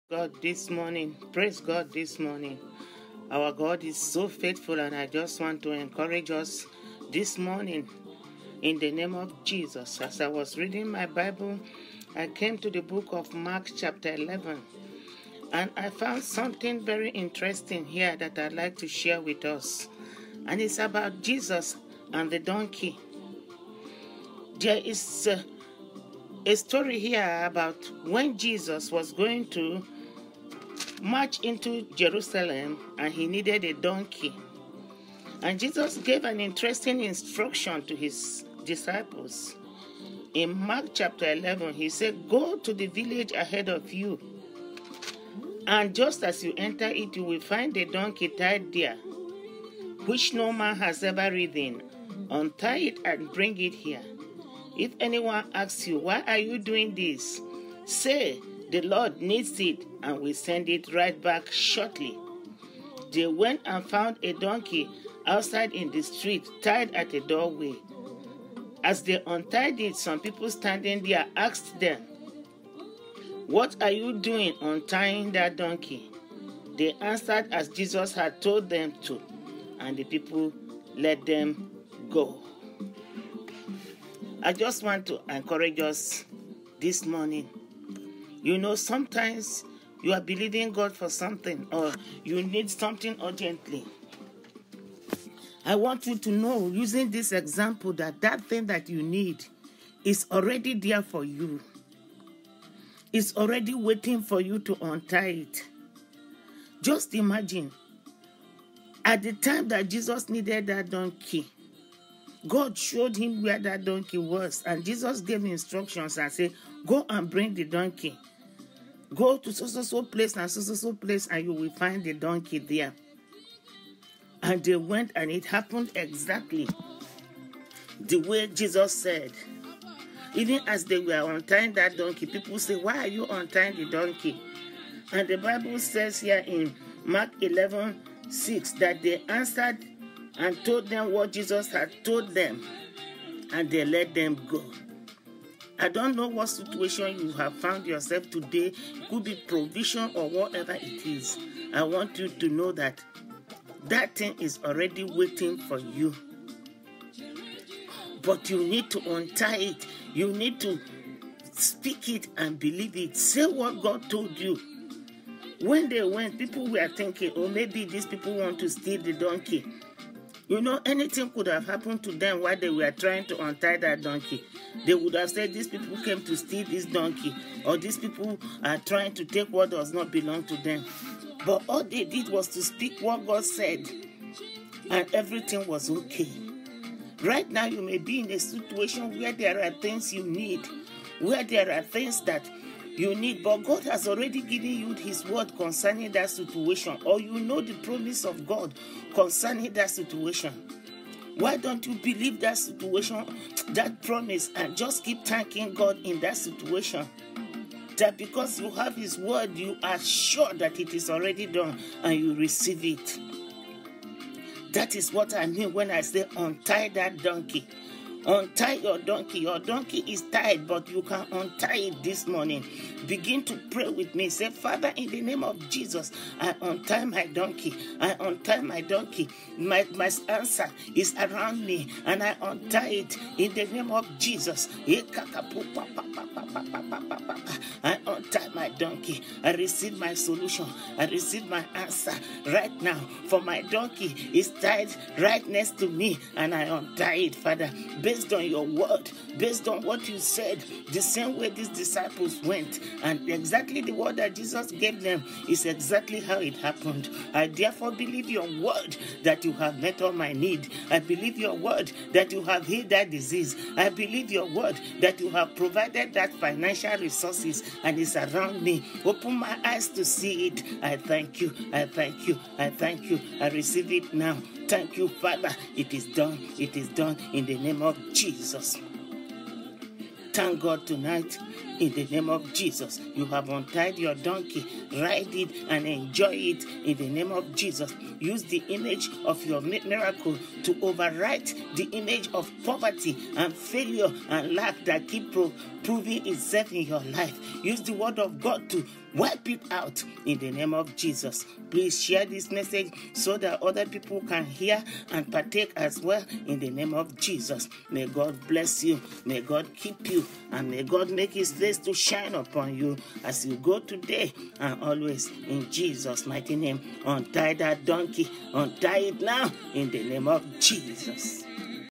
I UNTIE MY DONKEY! || BISHOP VERONICA ANUSIONWU
Are you facing a great need right now and don't know which way to turn? If your answer is yes, please find your answer in this message. Be encouraged for the Lord your God who is your good is with you. Please share also to encourage others in Jesus' mighty name. Amen.